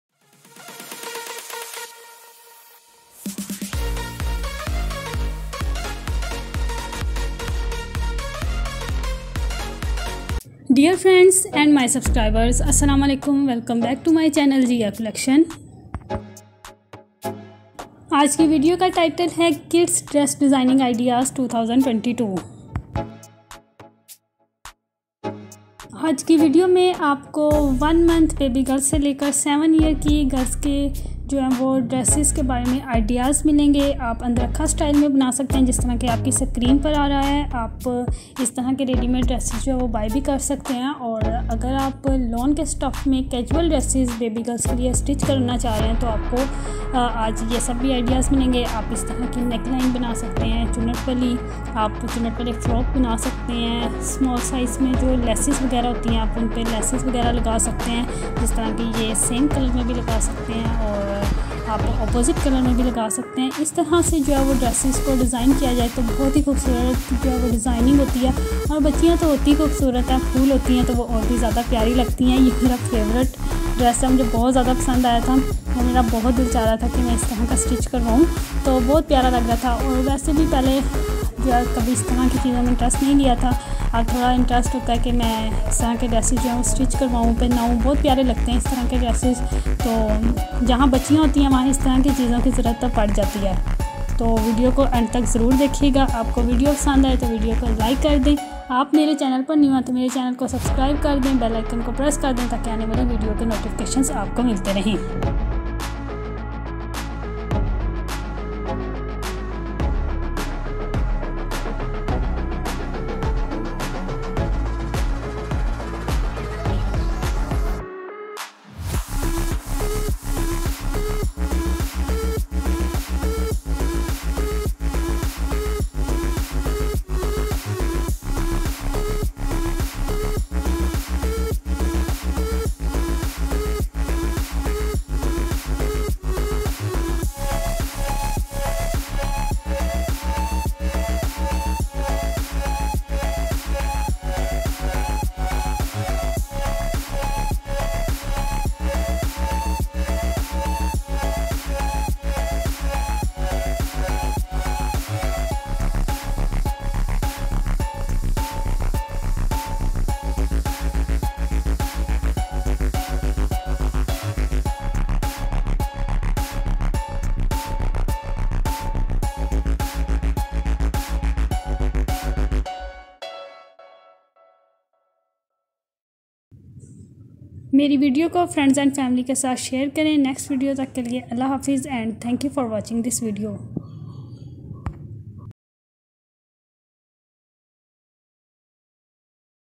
Kids dress designing ideas/Baby girls dresses/Kids Summer collection
Summer season ma ap apni baby girls k dresses ko trendy and beautiful kis trha se bna skti hn ye ideas mile gy is ley video ko end tk zaror dekhy. Meri video ko friends and family k sath share kry. Shukriya